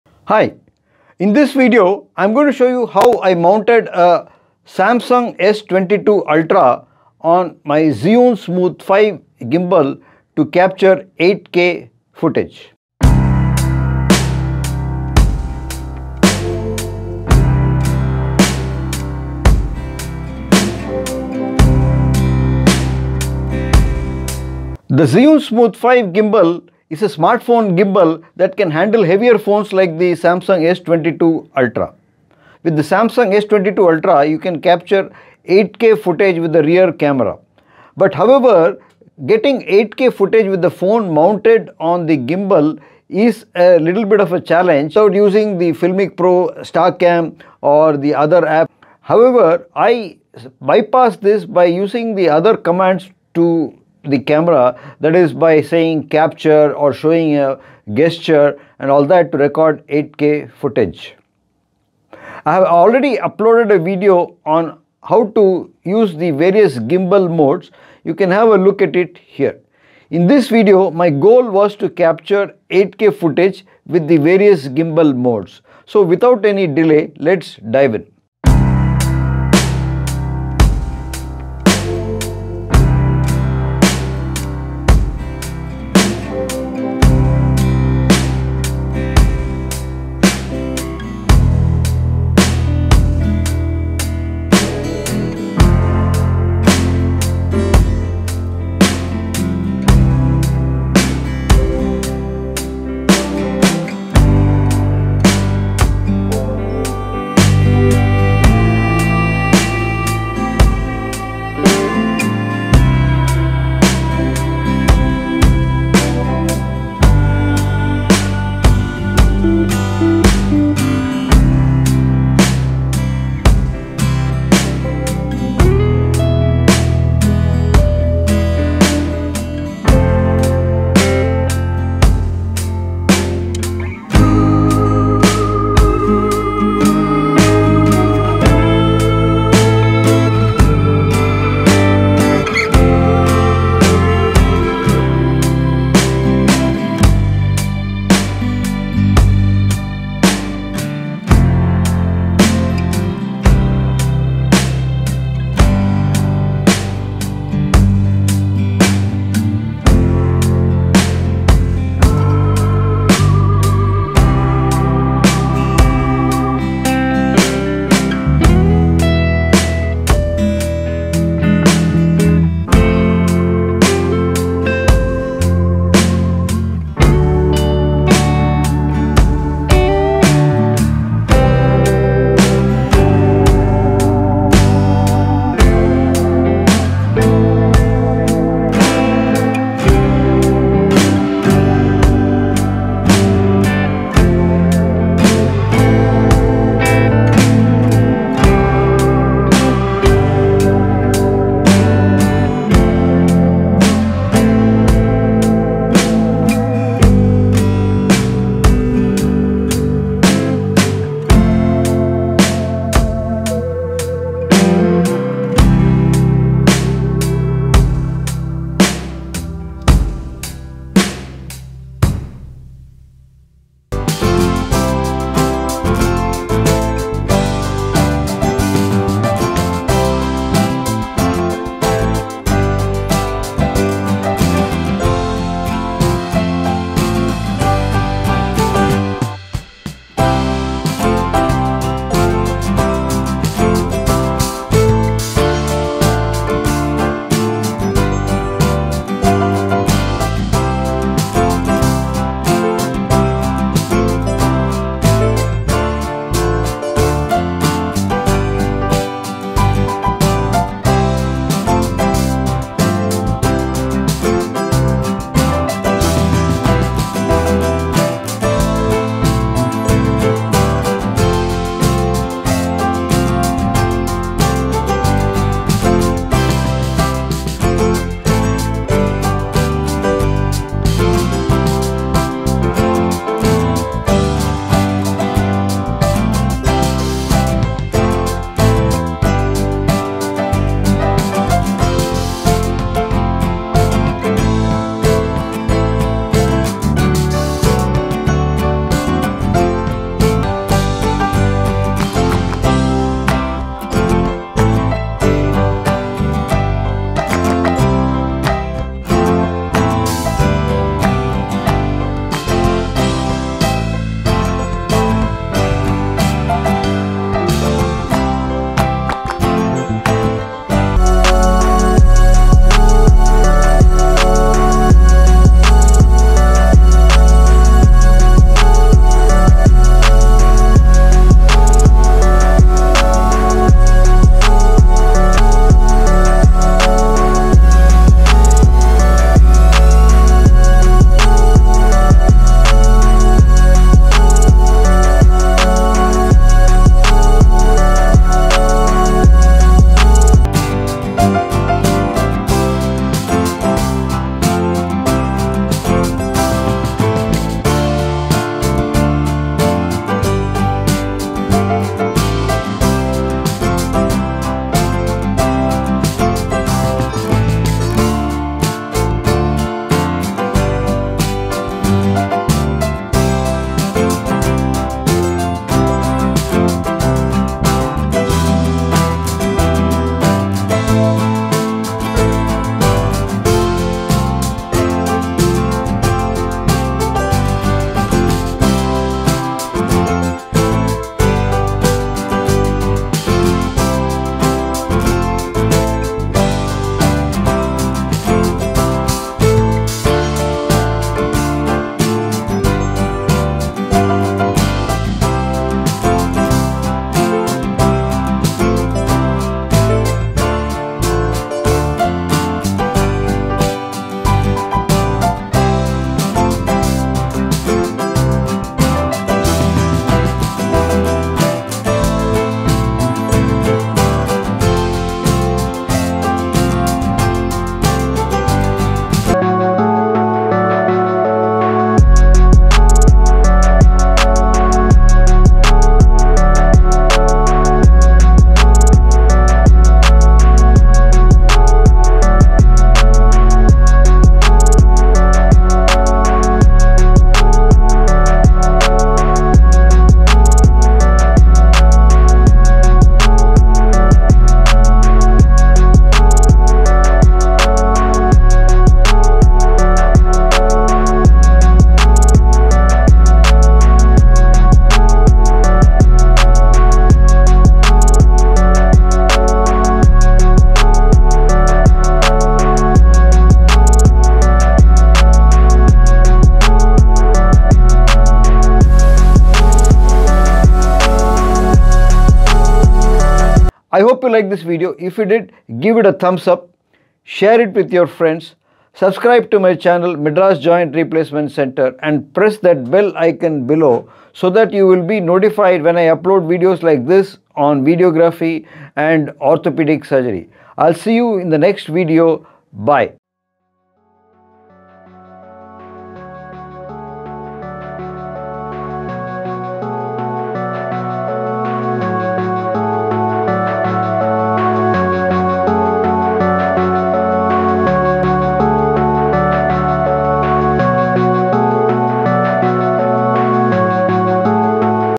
Zhiyun Smooth 5 gimbal moves Samsung S 22 Ultra 8K Video Camera
Zhiyun Smooth 5 Samsung gimbal moves S 22 ultra 8K Video Camera. Shoot high resolution 8K video with this combo.